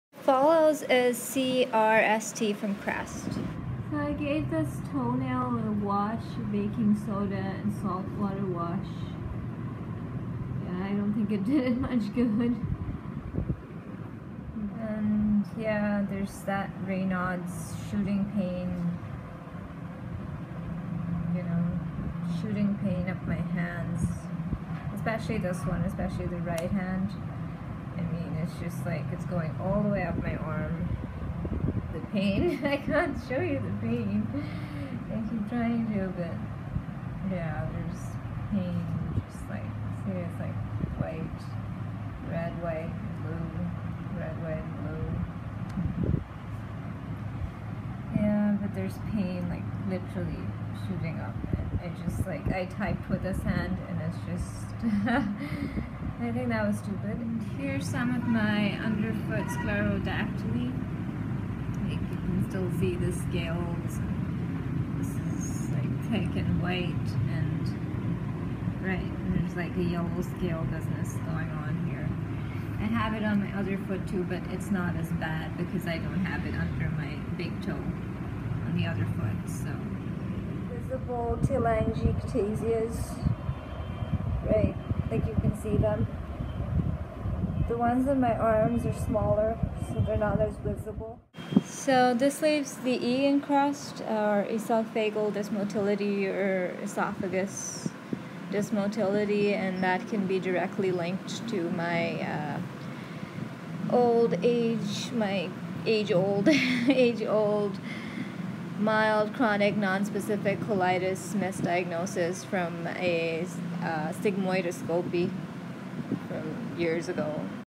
CREST syndrome Empirical Anagram Breakdown
Empirical examination of a silent rarely diagnosed disease; that usually goes unchecked.